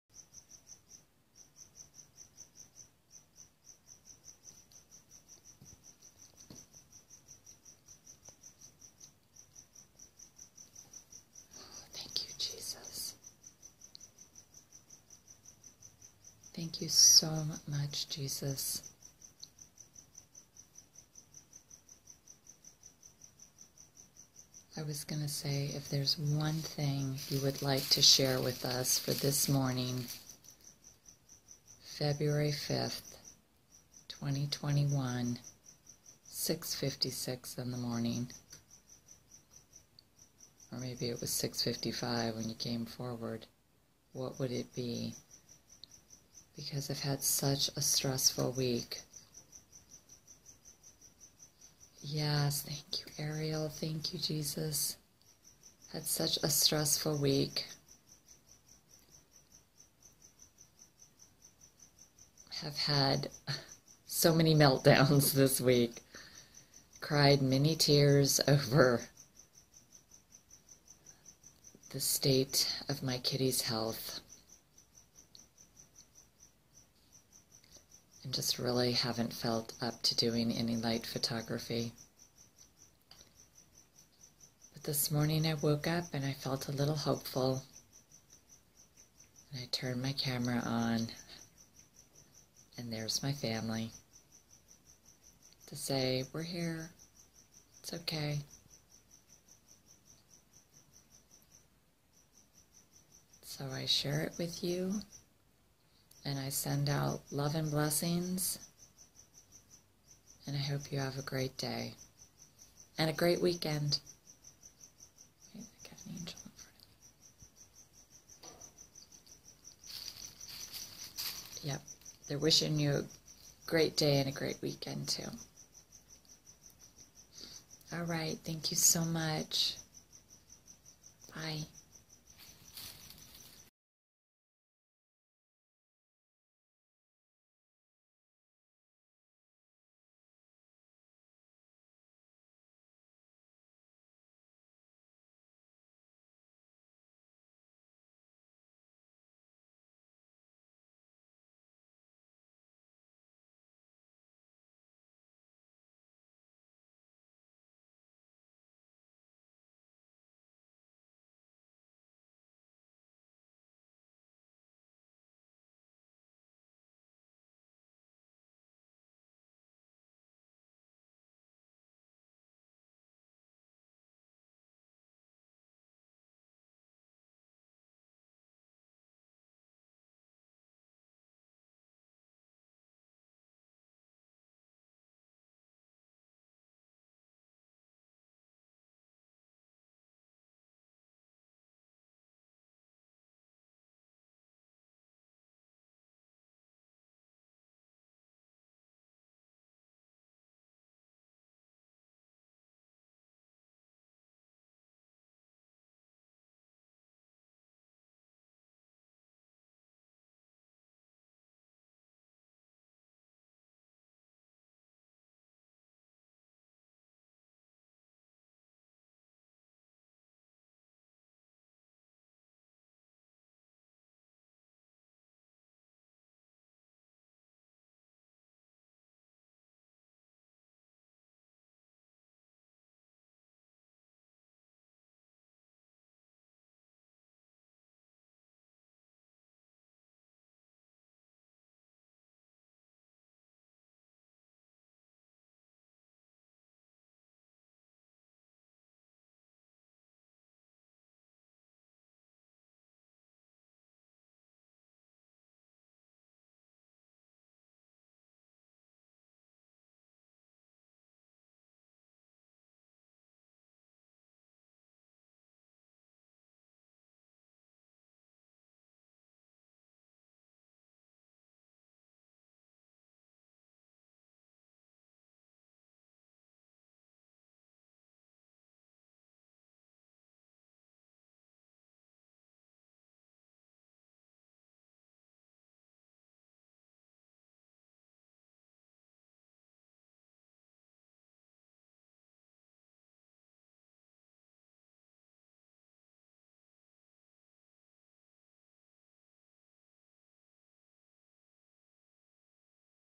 Love and Blessings for February 5, 2021
*NOTE: THIS VIDEO IS BEST VIEWED IN A DARK ROOM IN ORDER TO SEE THE LIGHT.

As I mentioned in this video, I had not been doing any Light work this week since I really did not feel up to it. I had received the blood results for my kitty,  Boo Boo, after I had taken him to see the veterinarian last Friday. The news was not good and, it knocked me off center. It took me a few days to absorb the news, see if Boo would respond favorably to the new medication (which he thankfully has) and to figure out how I wanted to personally approach the news. So, this morning I turned my camera on for the first time in a week and it was good to see my Light family come forward!

P.S. All prayers and positive thoughts for Boo Boo are welcome and much appreciated!